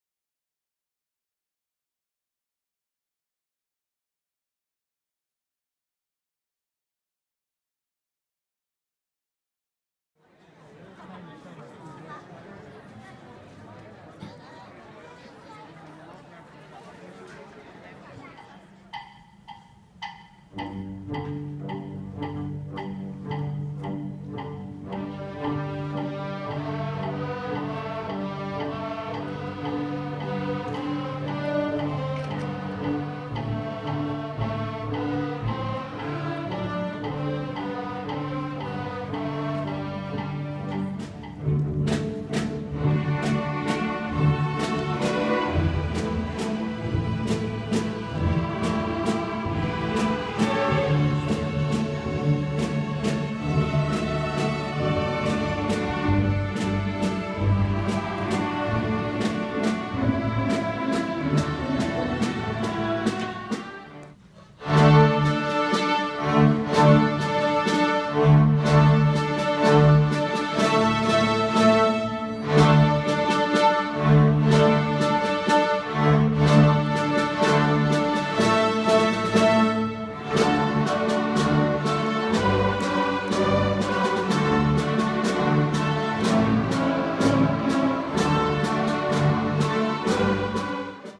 Jackson's Fall Concert 2015
District 102 Music Department's Fall Orchestra Concert
Thursday, November 12, 2015
Park Jr. High Auditorium